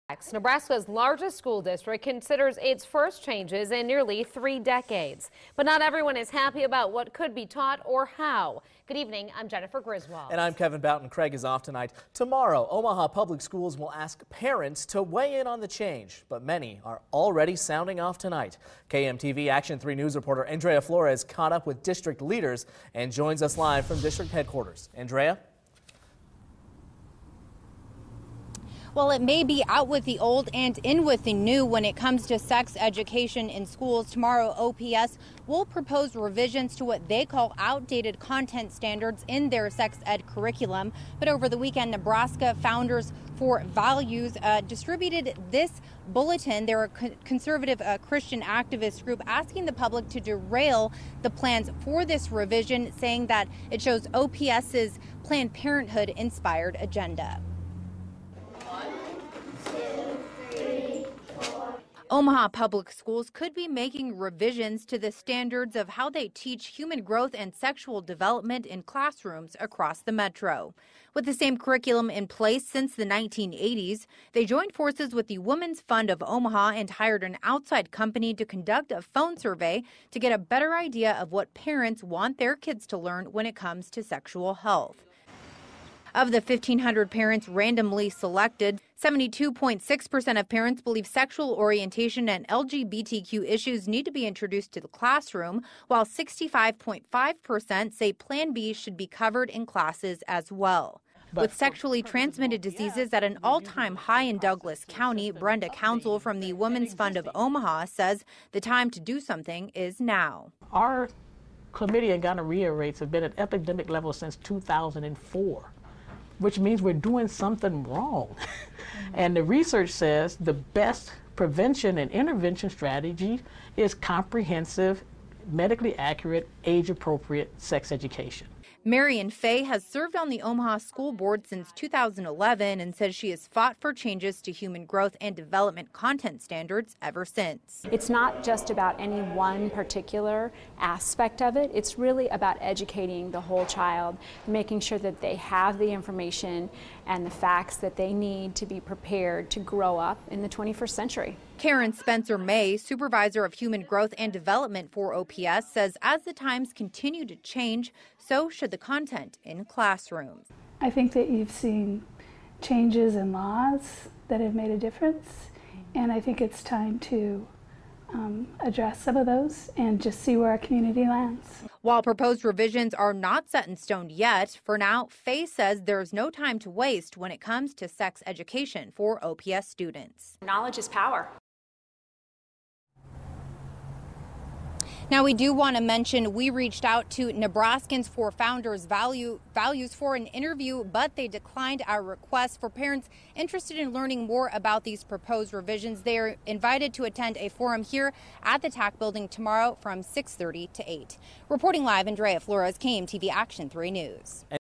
OPS to propose revisions to sex education content standards at forum
Omaha Public School will provide provisions to content standards when it comes to Human Growth and Development education.